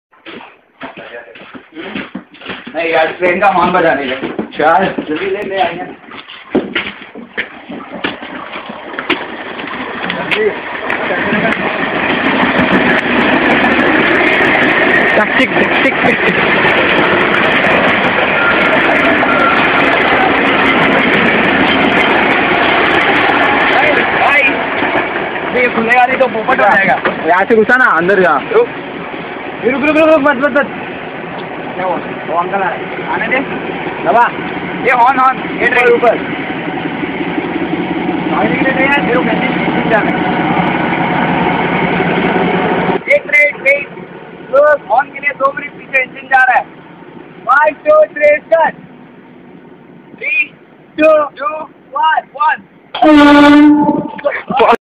Horn of Train
This video was taken whn i & my friend made a gr8 dearing 2 blow a trains horn of a train inside the shed behind my college.
But we did'nt went there for almost 5 days after that bcoz the RPF(Railway Police Force)inspector fled behind us for that but truly it was a gr8 fun frnds